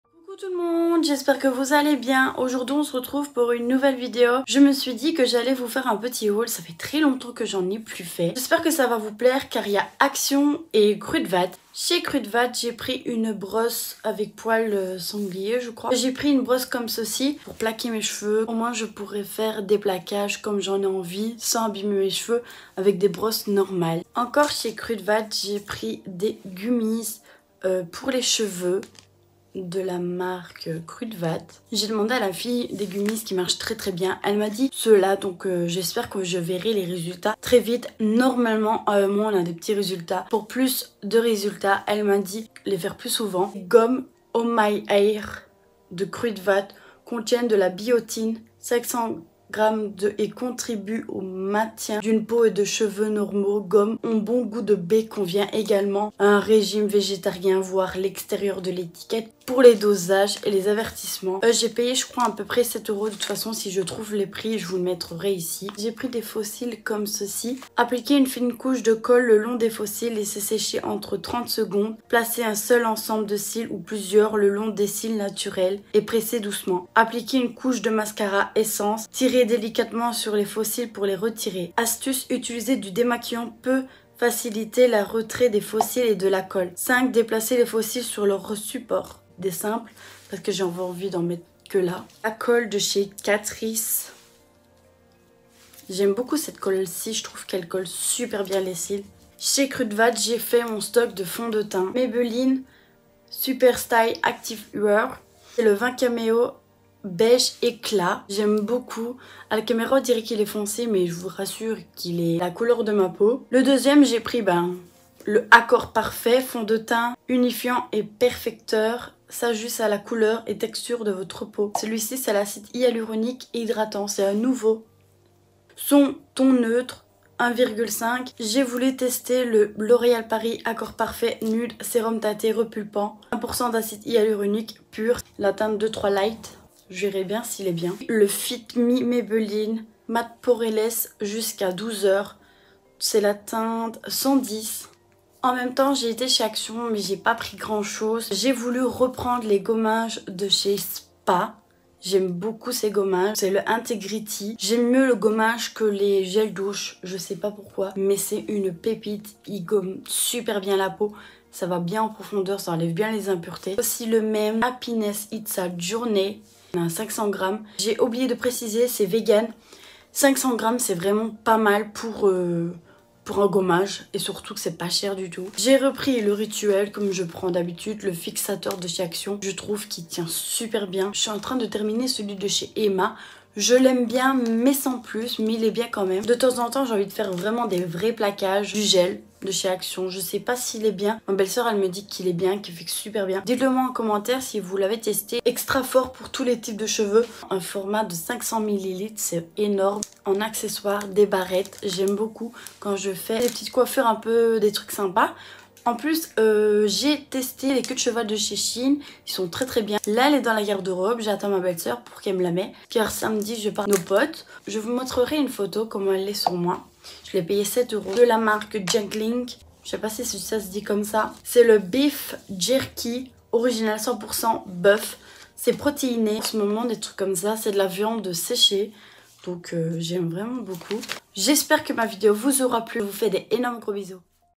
HAUL : kruidvat & action
Hello on se retrouve pour un petit haul

La brosse j’ai pas trouver le prix

j’ai pas trouver les faux cil exact

J’ai pas trouver ma teinte

Me contacter